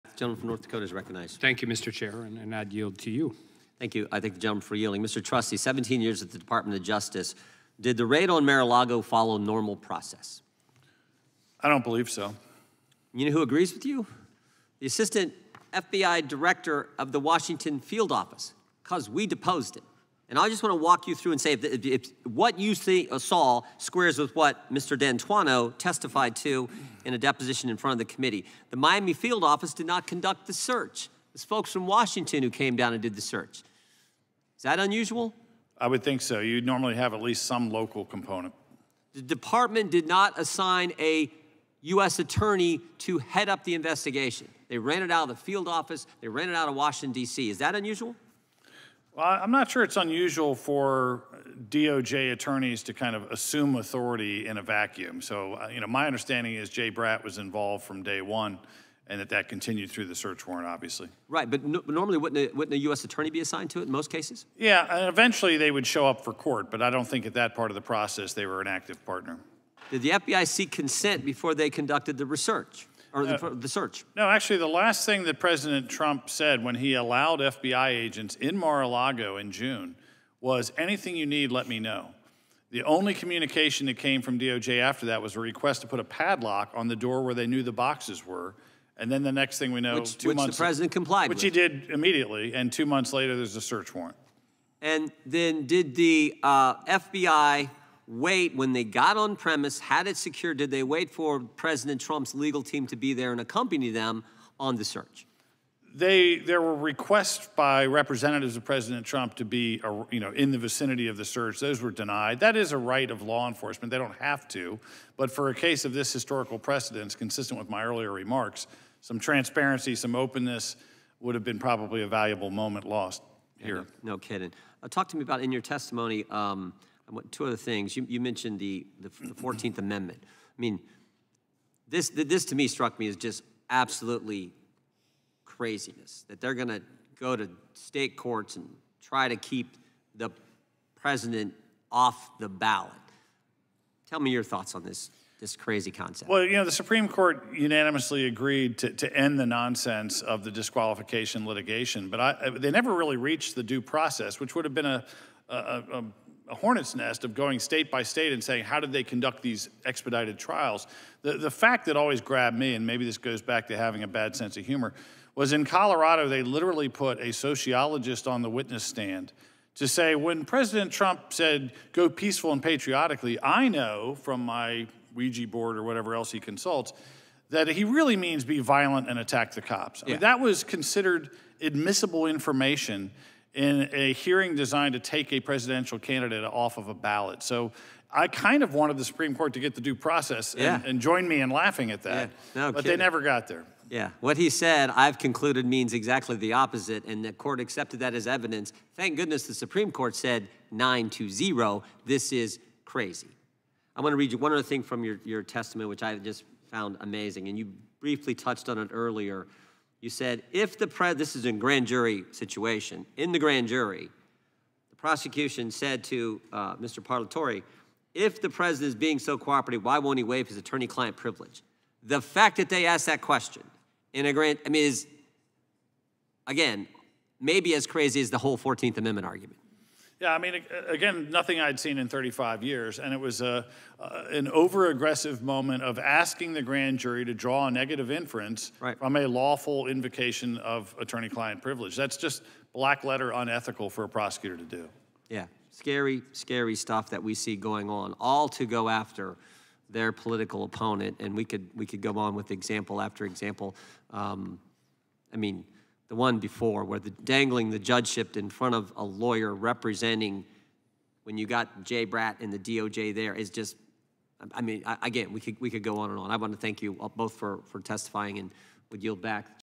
'Did The Raid On Mar-A-Lago Follow A Normal Process?': Jim Jordan Questions Ex-Federal Prosecutor
At today's House Weaponization of the Federal Government Committee hearing, Rep. Jim Jordan (R-OH) questioned former Federal Prosecutor James Trusty about the raid on Mar-a-Lago.